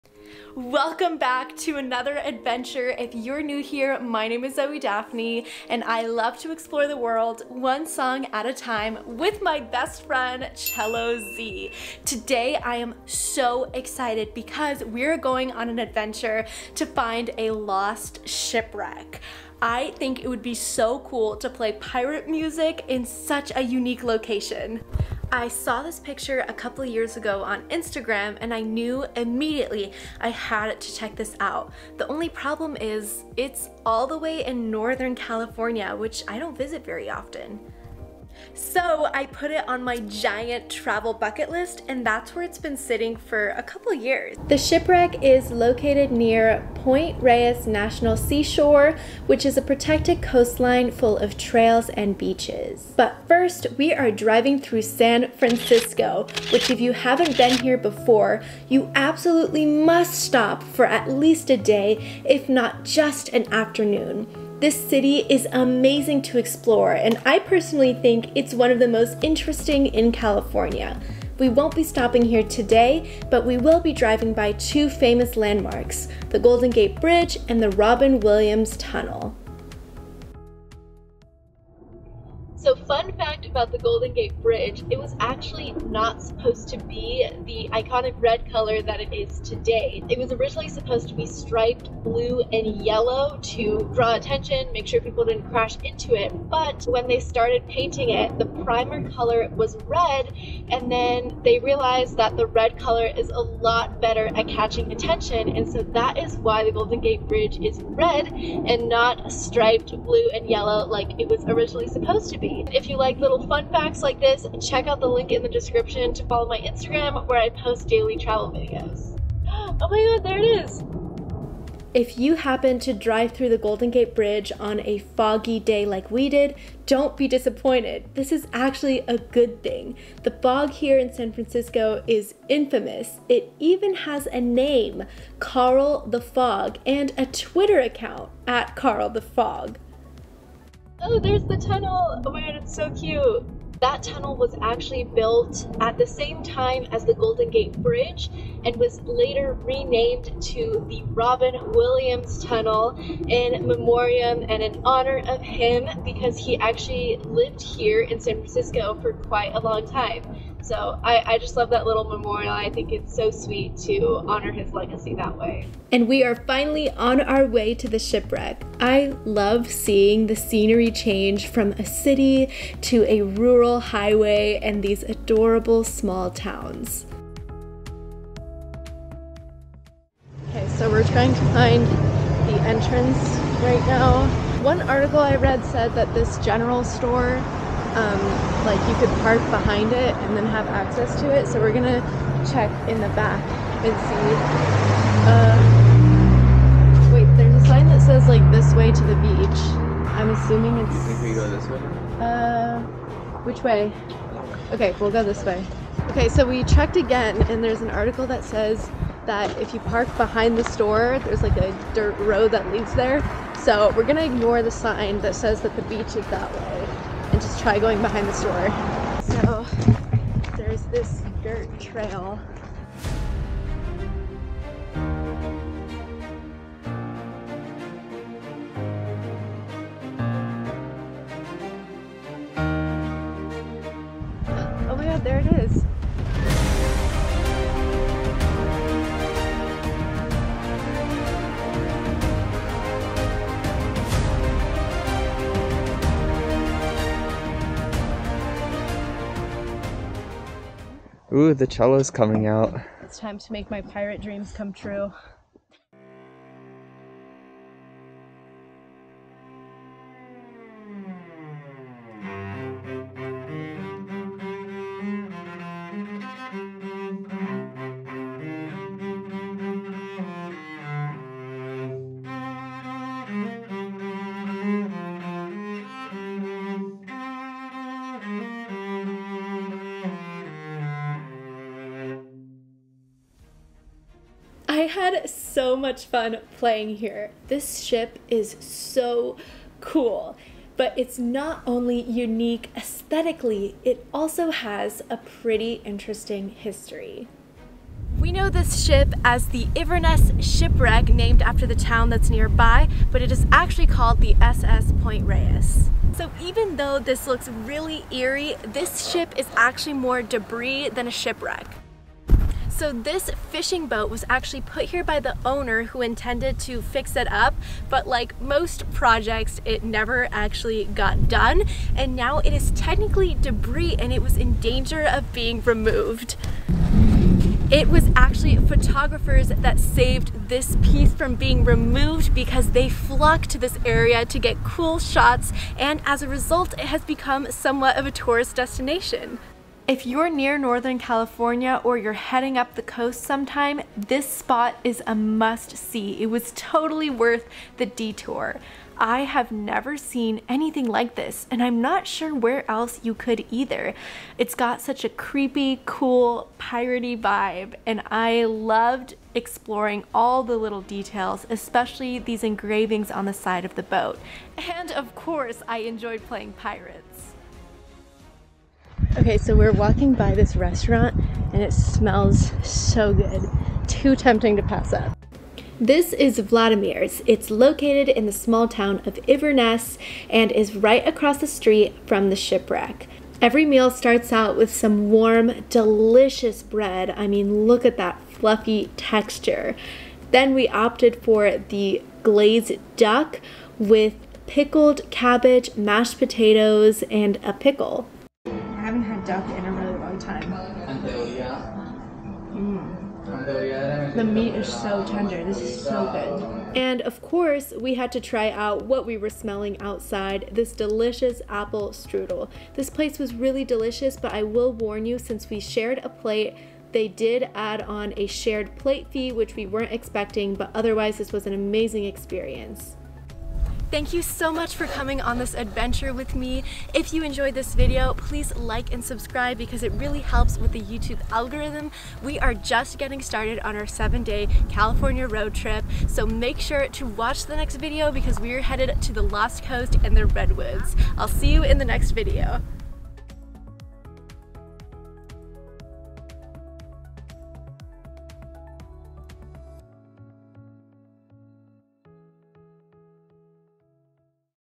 I can't believe this is in California!!! Point Reyes Shipwreck + San Francisco Road Trip
I'm seriously mindblown that you can find a shipwreck right in Northern California! The journey there was almost as fun as the shipwreck itself. We passed by the Golden Gate Bridge and Robin Williams tunnel before having a pirate adventure at the Point Reyes Shipwreck!

✨Amazon Storefront ✨